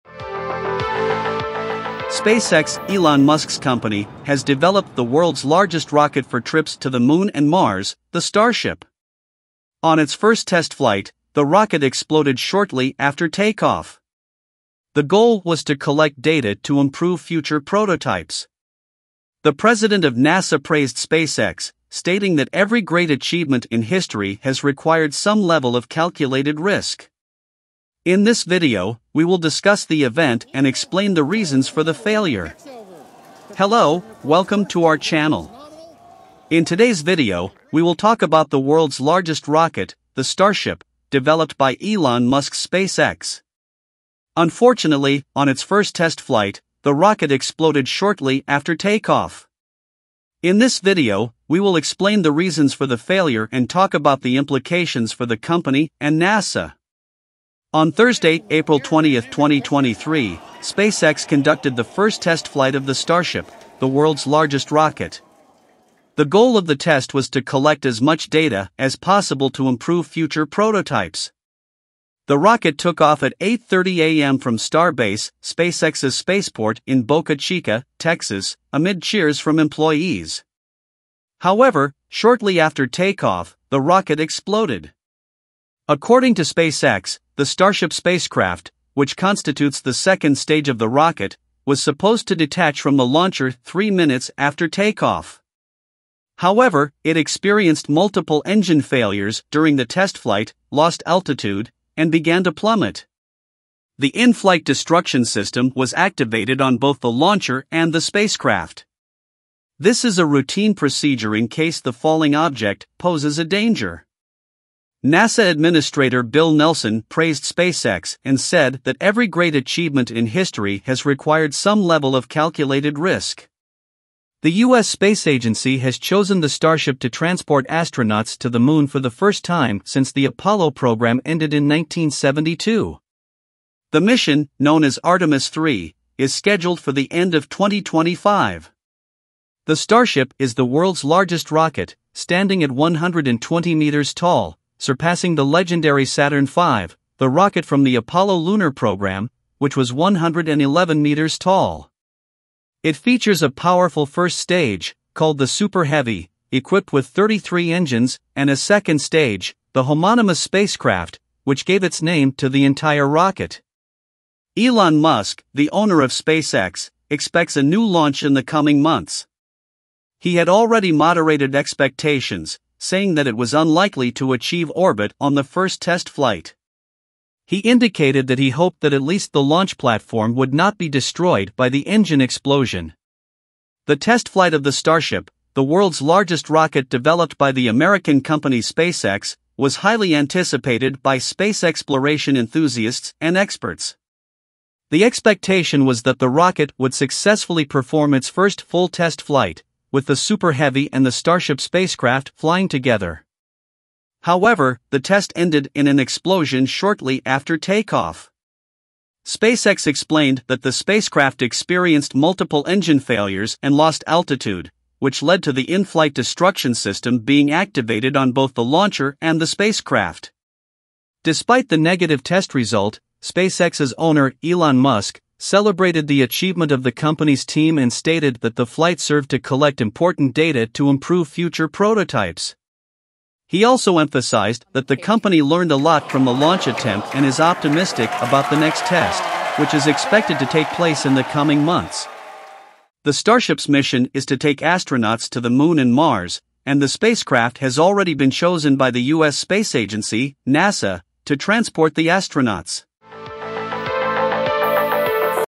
World's Largest Rocket, Starship, Explodes During Test SpaceX NEWS TODAY
SpaceX, Elon Musk's company, has developed the world's largest rocket for trips to the Moon and Mars, the Starship.

On its first test flight, the rocket exploded shortly after takeoff. The goal was to collect data to improve future prototypes.

The president of NASA praised SpaceX, stating that every great achievement in history has required some level of calculated risk. In this video, we will discuss the event and explain the reasons for the failure.